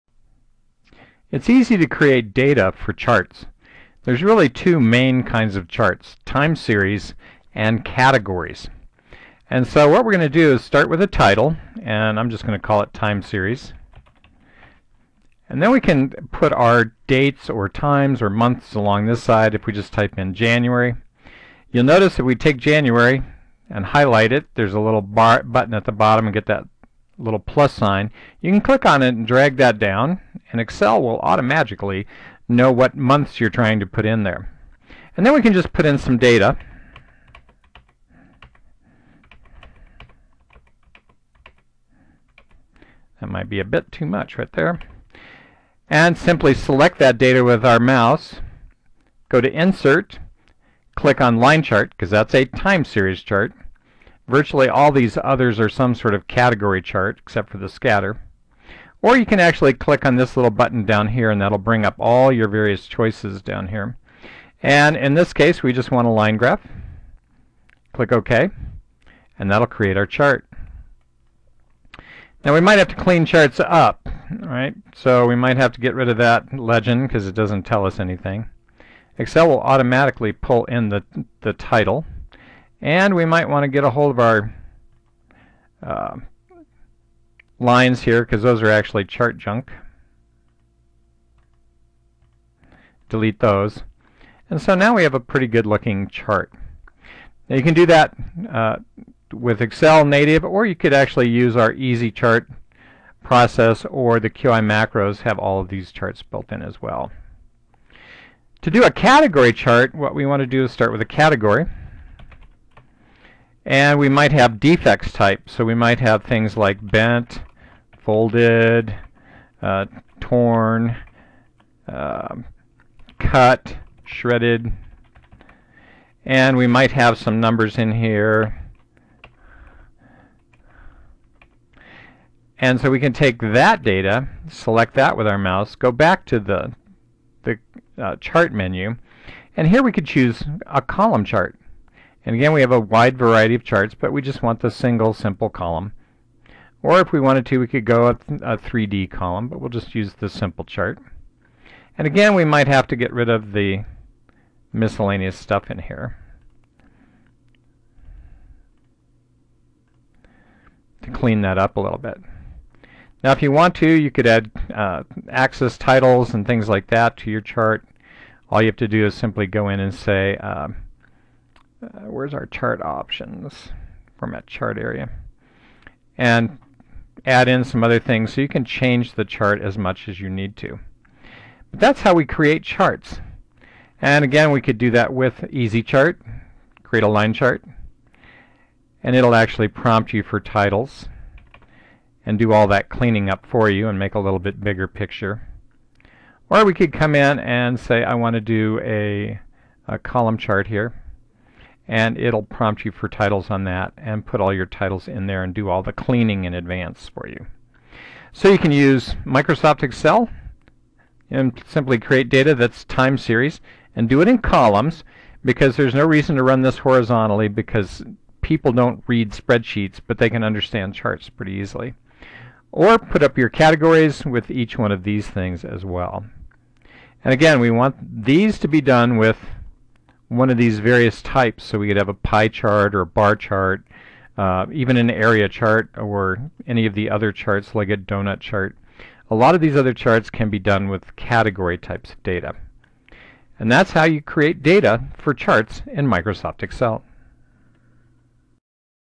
Creating Spreadsheet Data for Excel Charts
How to create data in an Excel spreadsheet for line, bar, column and pie charts charts.